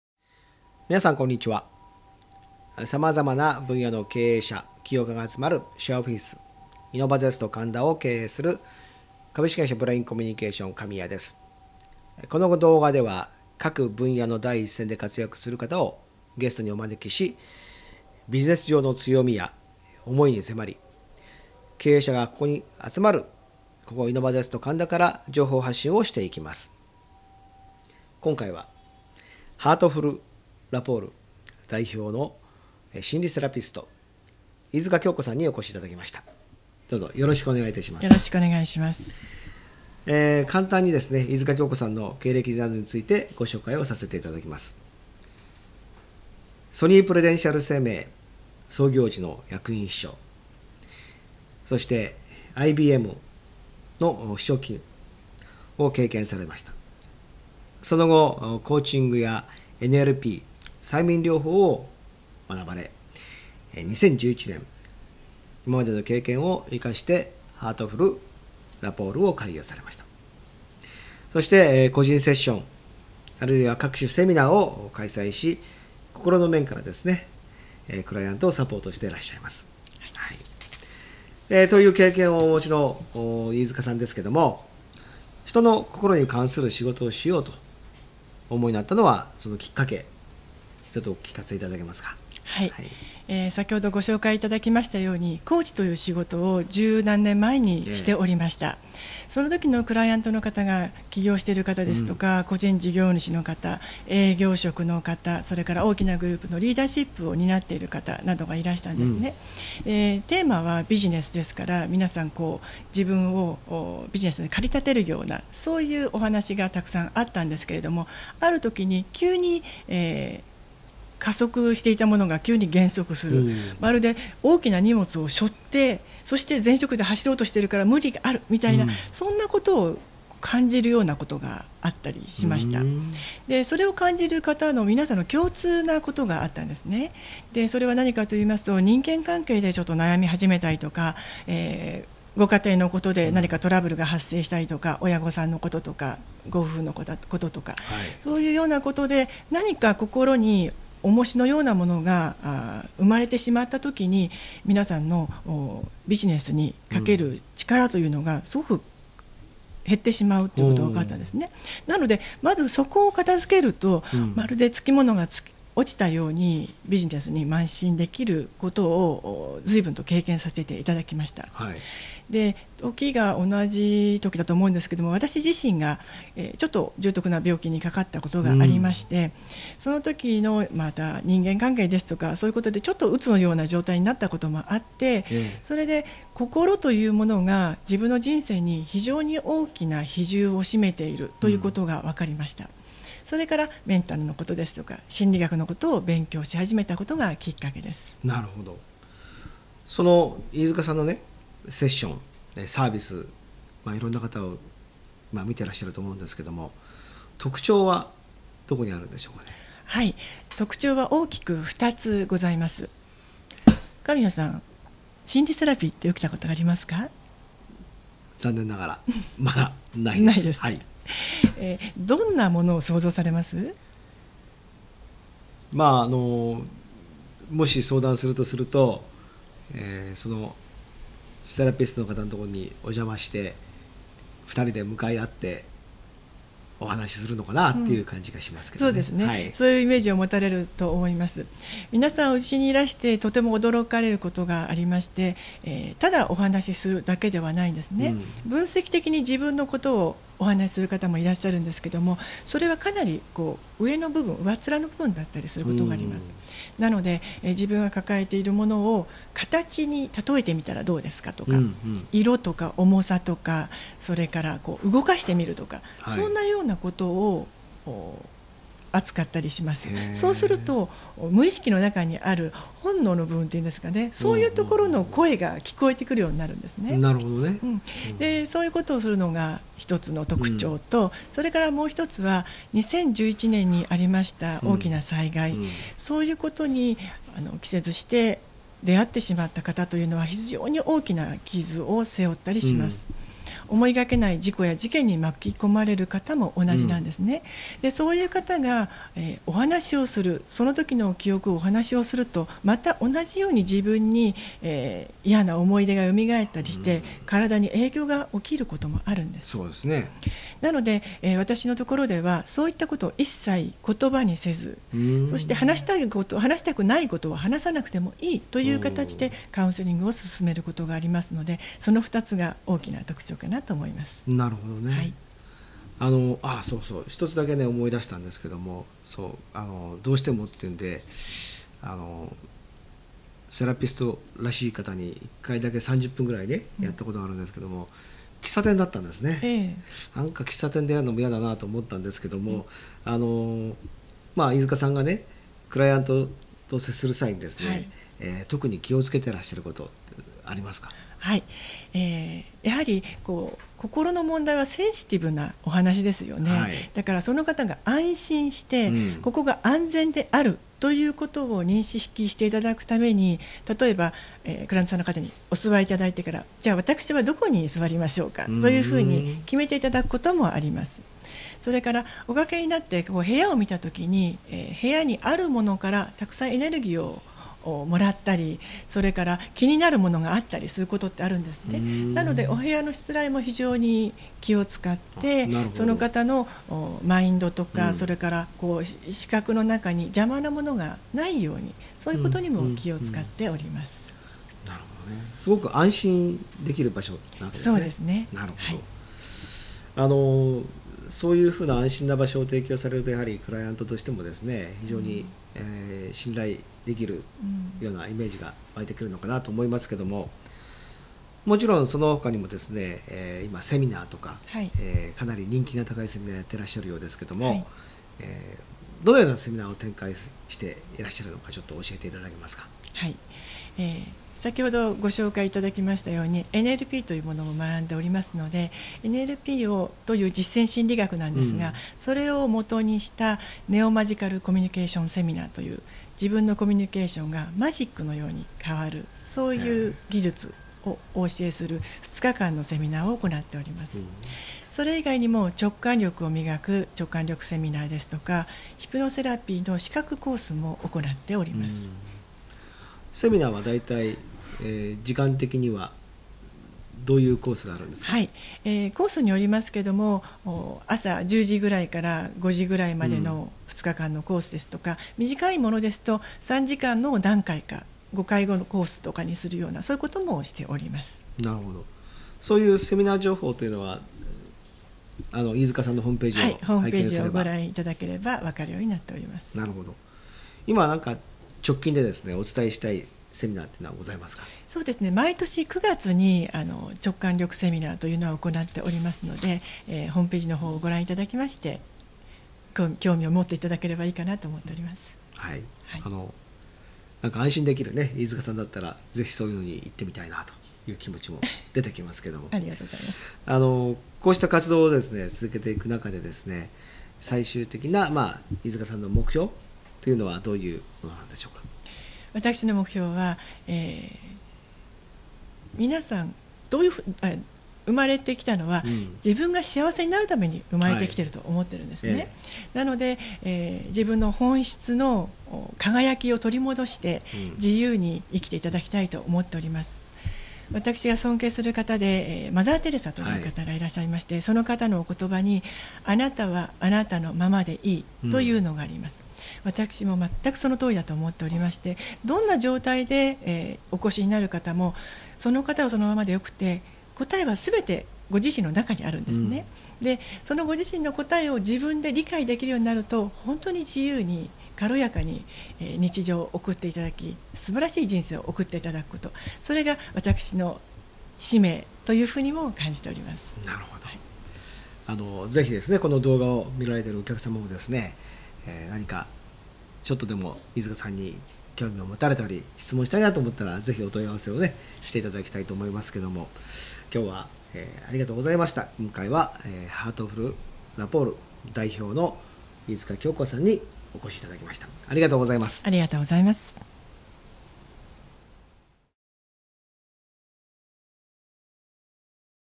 イノバゼスト神田よりビジネスの情報発信 ゲスト・飯塚京子さん
ハートフルラポールを運営する心理セラピスト、飯塚京子さんがゲストです。心理セラピーに対するイメージを覆すような、驚きの手法で、人々の心をエレガントにサポートします。